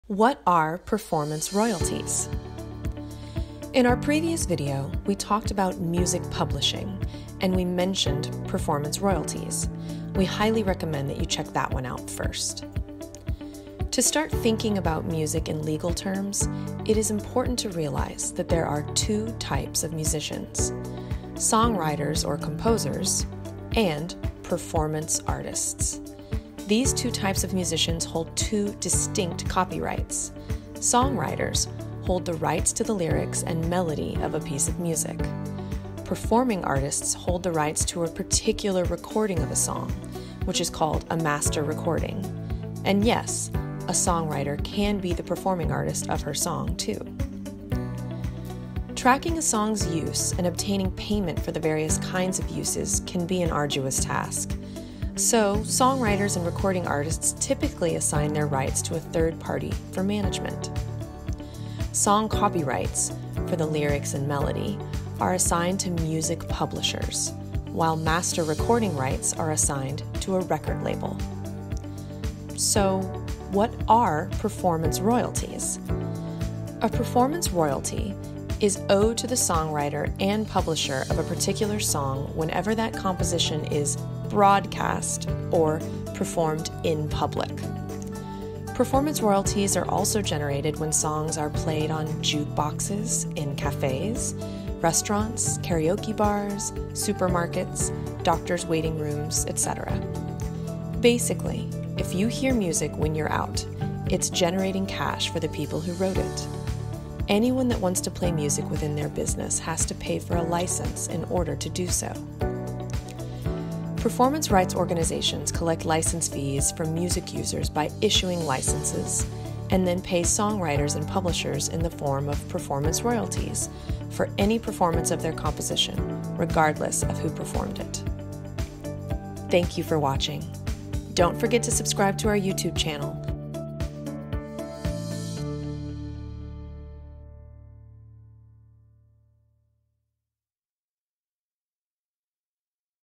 What are Performance Royalties and Performance Rights Organizations?
A "performance royalty" is owed to the songwriter and publisher of a particular song whenever that composition is “broadcast” or performed “in public.”

Performance Rights Organizations collect license fees from music users by issuing licenses, and then pay songwriters and publishers in the form of performance royalties for any performance of their composition.

Sesac, Licensing, Music Industry, Music Publishing, Ascap, soundexchange, entertainment law, harry fox agency, sync licensing, performing rights organization